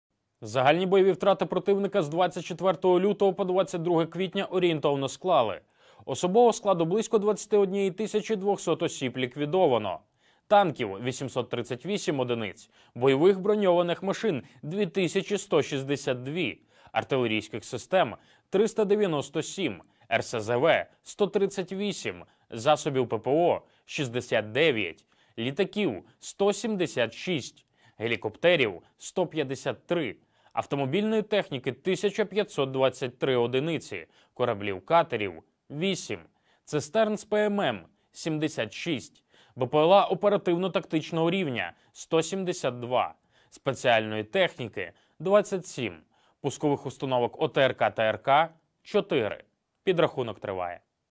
Загальні бойові втрати противника з 24.02 по 22.04
Загальні бойові втрати противника з 24.02 по 22.04 орієнтовно склали :
бойових броньованих машин ‒ 2162 од,
артилерійських систем – 397 од,
РСЗВ  - 138 од,
засоби ППО  - 69 од,
літаків  – 176 од,
гелікоптерів  – 153 од,
автомобільної техніки  - 1523 од,
кораблі /катери  - 8 од,
цистерн з ПММ  - 76,
Пускові установки ОТРК/ТРК  - 4.